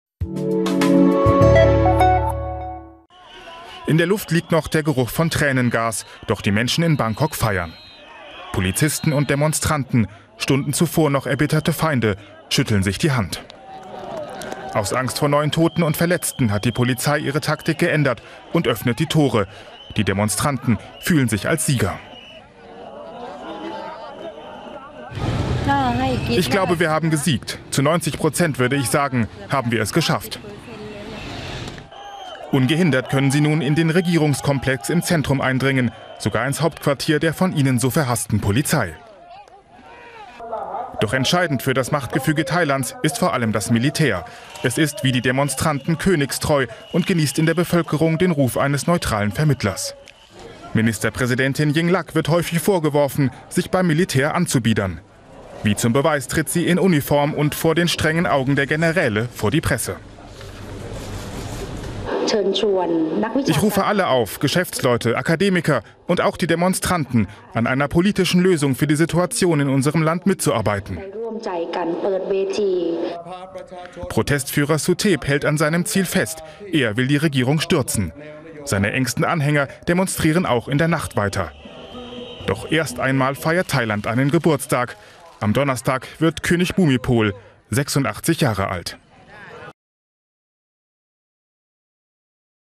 Bangkok: Polizei gibt nach | Journal
Bangkok: Statt Straßenschlachten und Barrikaden herrscht freie Bahn zum Regierungsgebäude und zur Polizei-zentrale. Es gibt nichts mehr zu stürmen für die Gegner von Regierungschefin Shinawat. Sie wollen zwar weiter protestieren, aber die Regierung hat wertvolle Zeit gewonnen.